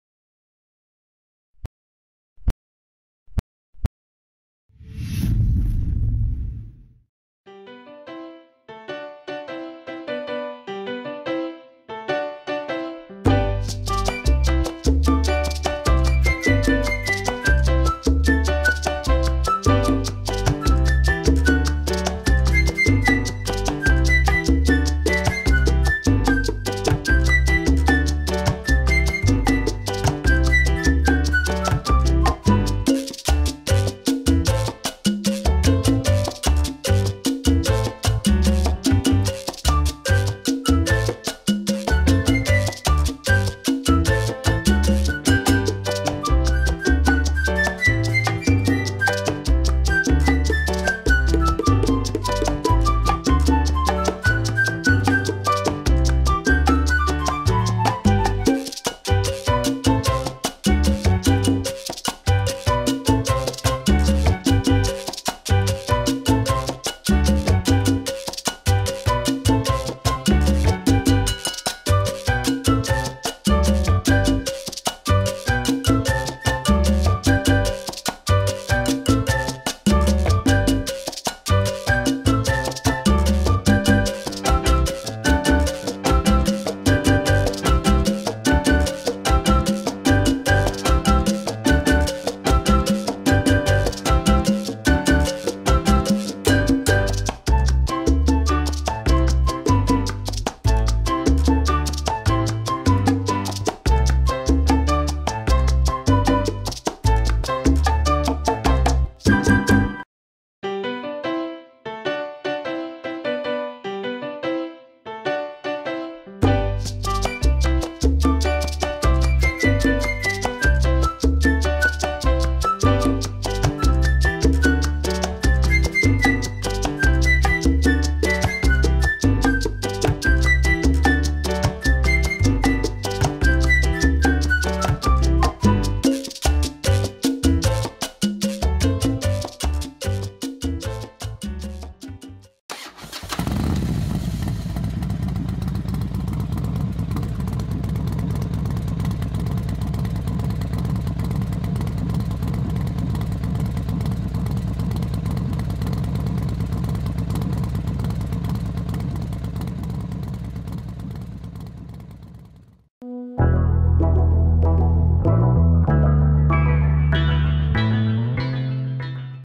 3D Trick Art on Paper Volkswagen Beetle
Amazing anamorphic illusion.
3D art on paper.
Speed drawing.
Volkswagen Beetle

Used materials:
Black markers
Eraser

Materiais usados:
Papel A4 180g
Lápis de carvão
Marcadores pretos
Borracha
Lápis de pastel seco